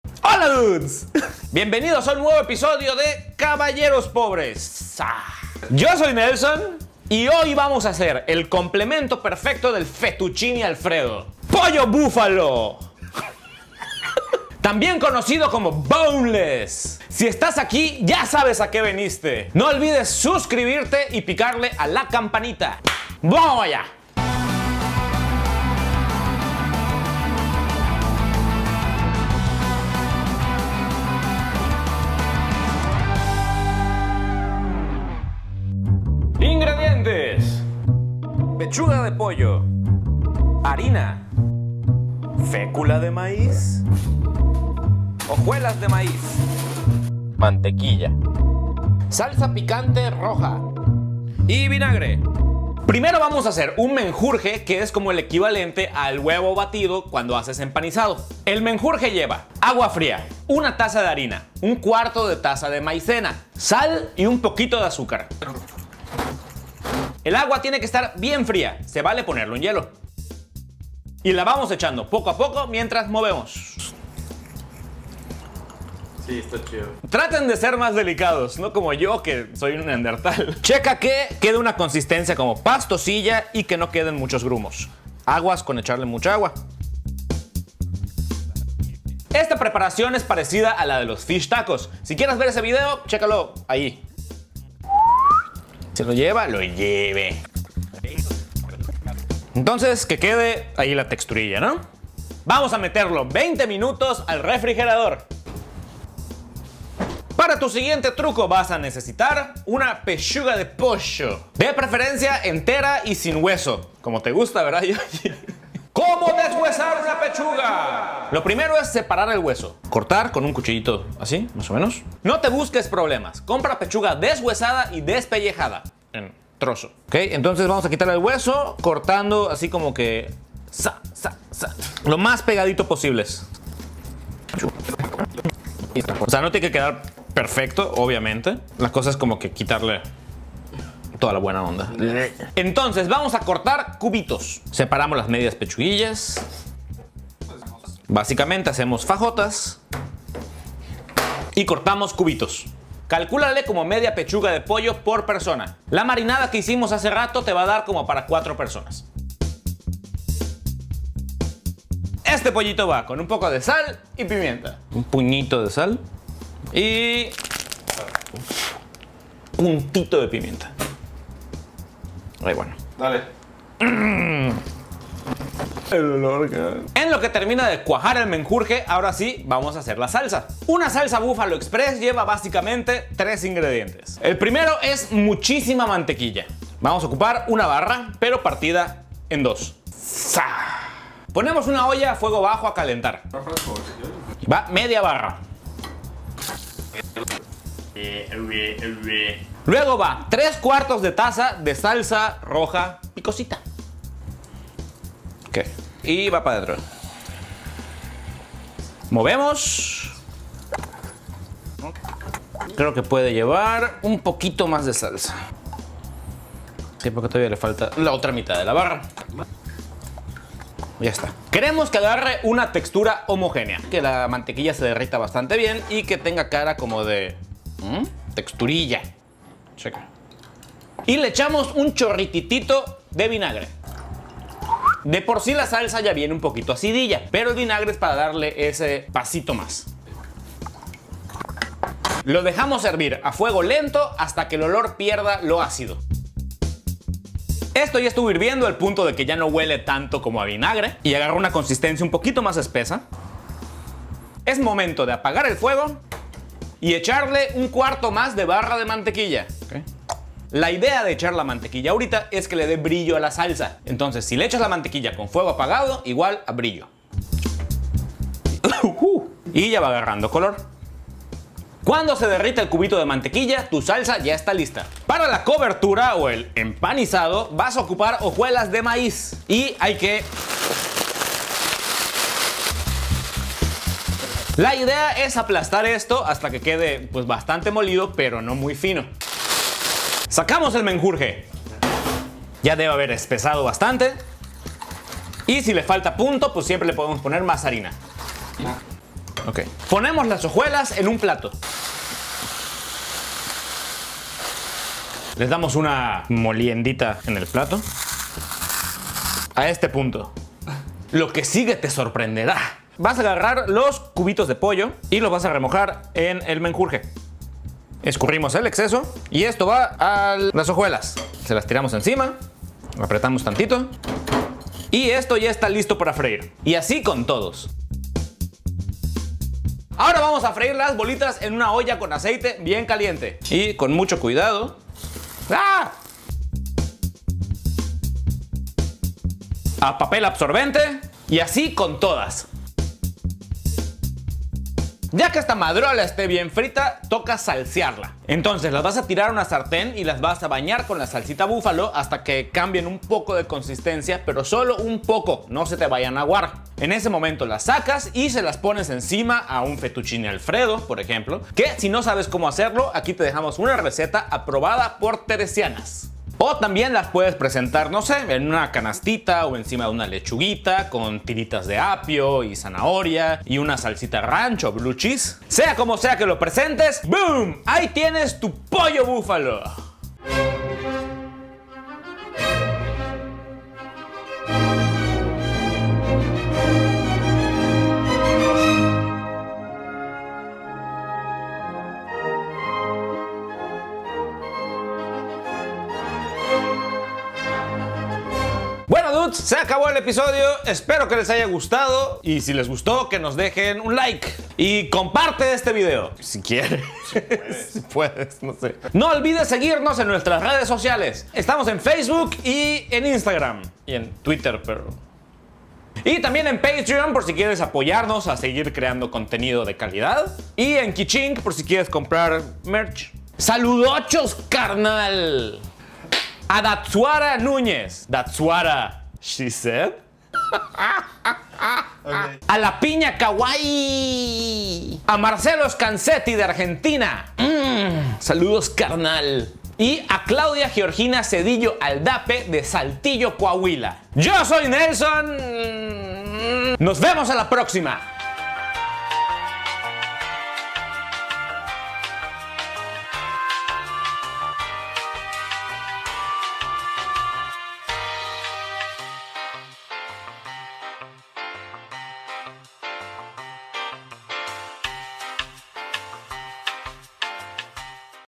POLLO BÚFALO ! BONELESS !!! CABALLEROS POBRES
Cómo preparar POLLO BÚFALO 🐔🐃 estilo BONELESS ! fácil, rapidísimo y con pocos ingredientes sencillos! 💯
⬇️ Síguenos ⬇️